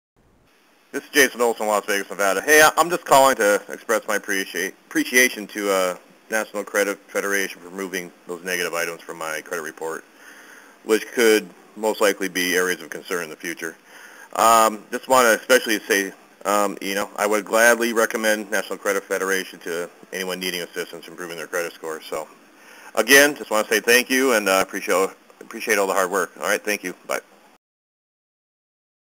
Jason would gladly recommend NCF to anyone who needs help with their credit
This is Jason in Las Vegas, NV. Hey, I'm just calling to express my appreciation to National Credit Federation for removing the negative items from my credit report which could be an area for concern in the future. I just want to especially say I would gladly recommend National Credit Federation to anyone need assistance improving their credit scores. Again, I just want to say thank you and I appreciate all the hard work. Thank you, bye.